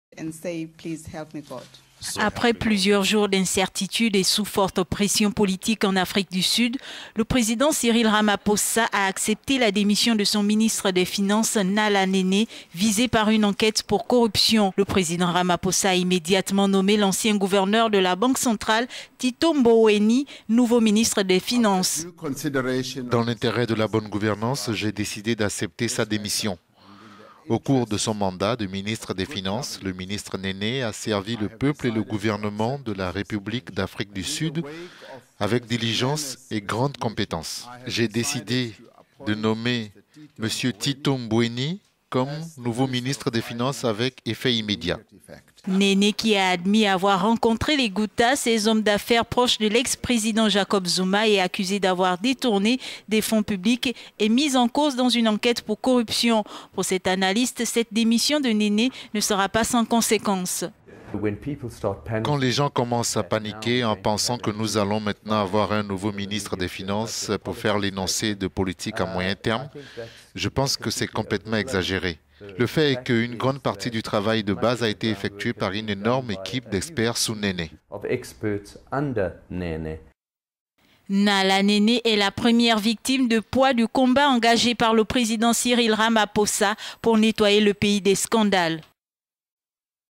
Afrique du Sud : nomination d'un nouveau ministre des Finances
*Après plusieurs jours d'incertitudes et sous forte pression politique en Afrique du Sud, le président Cyril Ramaphosa a accepté la démission de son ministre des Finances, Nhlanhla Nene visé par une enquête pour corruption*.

Le président Ramaphosa a immédiatement nommé l'ancien gouverneur de la banque centrale Tito Mboweni , nouveau ministre des Finances.

" Dans l'intérêt de la bonne gouvernance, j'ai décidé d'accepter sa démission. Au cours de son mandat de ministre des Finances, le ministre…